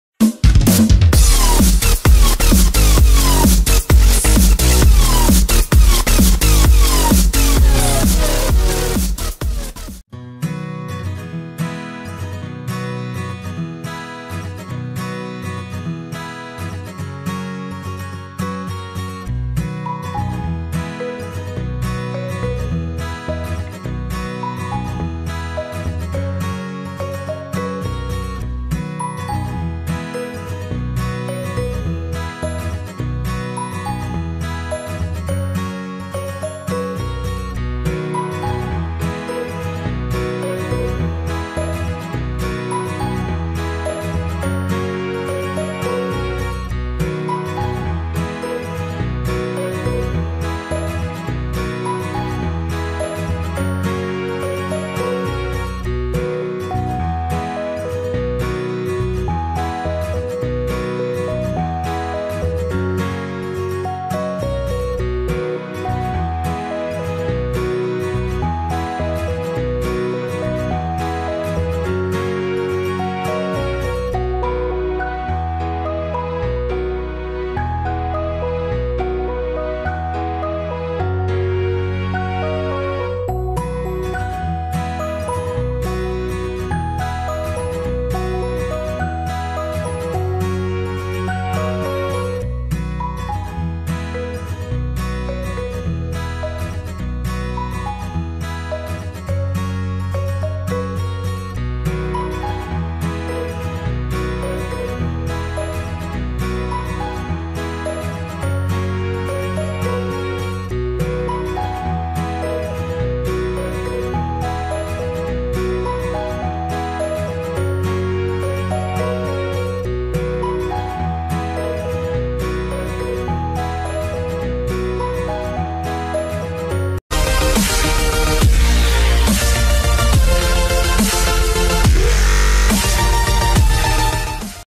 Sarah Nowak: Ihre „kleine Prinzessin“ ist jetzt auf der Welt
Wunderbare Nachrichten von Sarah Nowak: Die stolze Mama hat am Montag ein Töchterchen zur Welt gebracht, wie sie nun selbst verriet.
Sarah Nowak (26) und ihr Mann Dominic Harrison sind seit Montag stolze Eltern! Das Playmate hat am 27. November um 08:30 Uhr seine „kleine Prinzessin“ zur Welt gebrach ...